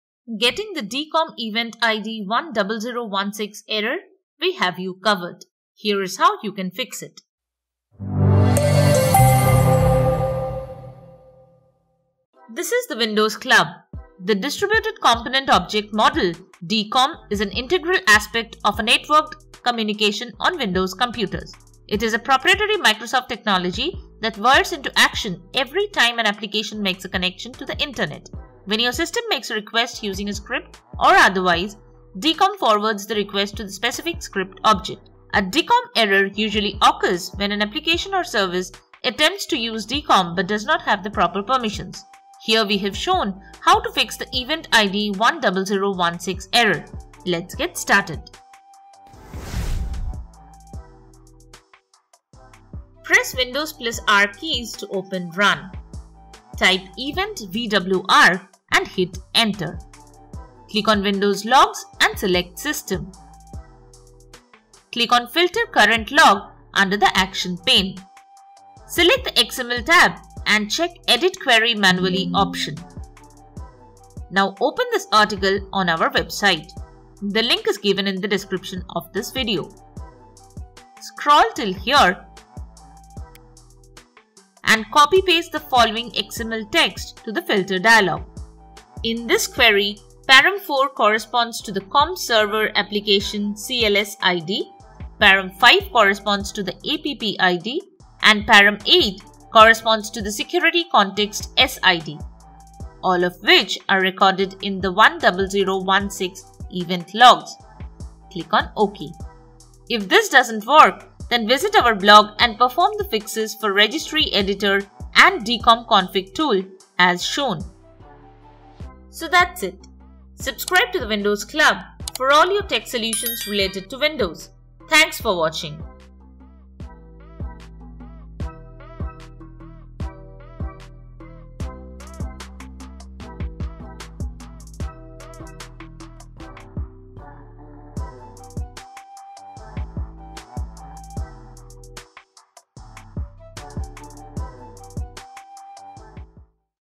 How to fix DCOM Event ID 10016 error on Windows 11/10
The Distributed Component Object Model (DCOM) is an integral aspect of networked communication on Windows computers. It is a proprietary Microsoft technology that whirs into action every time an application makes a connection to the internet. A traditional COM can only access information on the same machine, whereas DCOM can access data on remote servers.

In this tutorial, we will identify the cause and then provide the possible solutions to the issue of DCOM (DistributedCOM) Event ID 10016 error that might show up in the Windows event viewer in the course of normal operations of Windows 11/10.